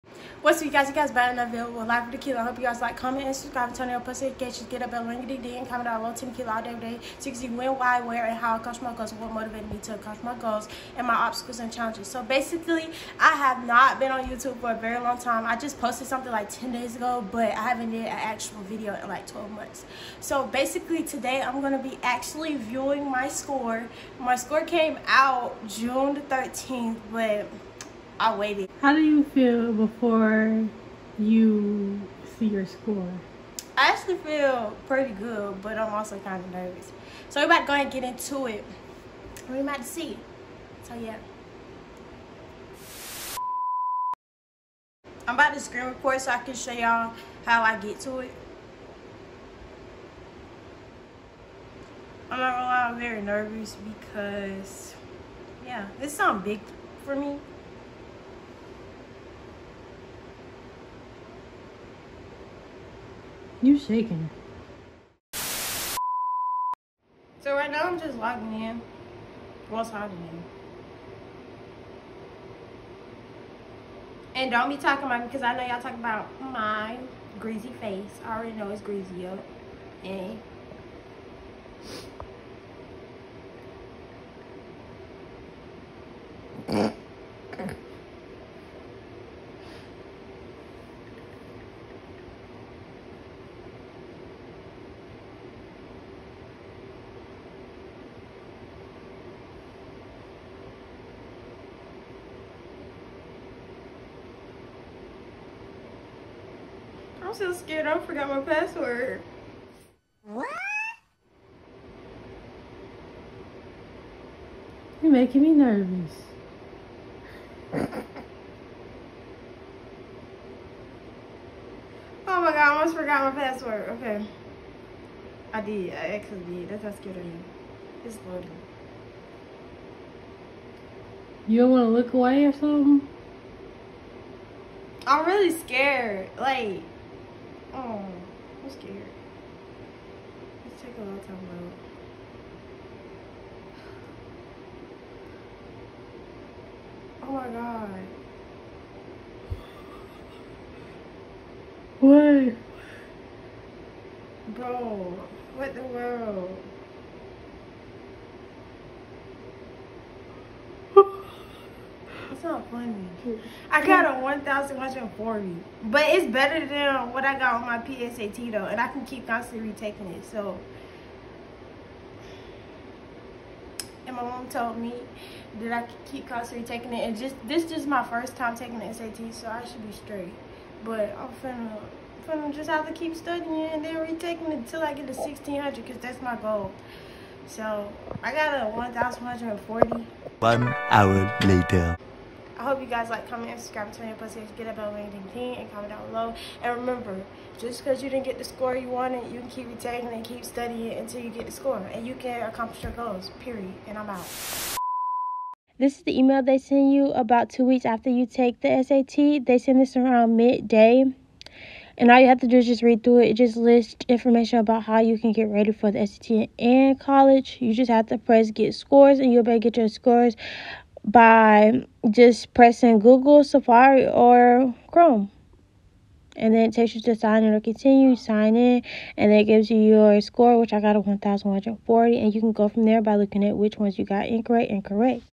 SAT Score Reaction (June 2023)😢😢😢😢
In this video I’m showing and telling you how I felt before and after viewing my SAT score. This is my first time taking the SAT.